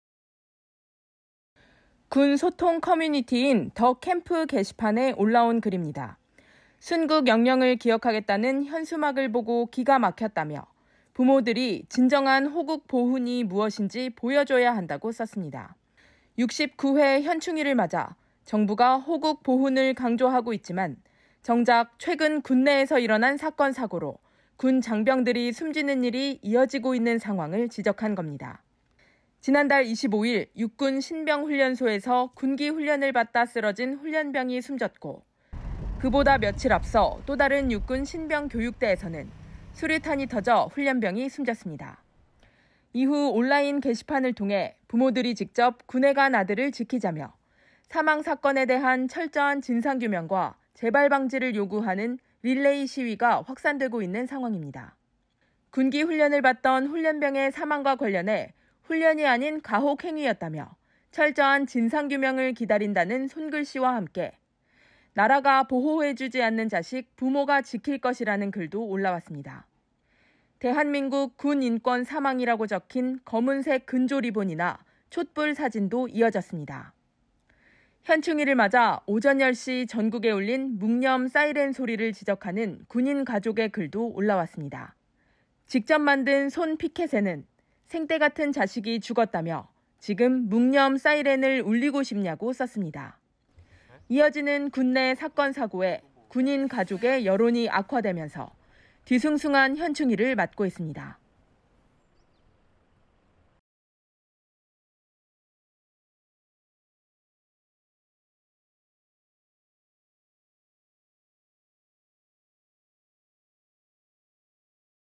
"지금 묵념 사이렌 울리고 싶냐" SNS 시위 확산에 뒤숭숭한 현충일｜지금 이 뉴스
최근 군내에서 발생한 사건 사고로 뒤숭숭한 현충일을 맞았습니다. 군 소통 커뮤니티인 '더캠프' 게시판에는 현충일인 오늘(6일) '군 인권 사망' 적힌 근조리본과 촛불사진 릴레이로 올리는 일종의 SNS 시위 확산되고 있습니다.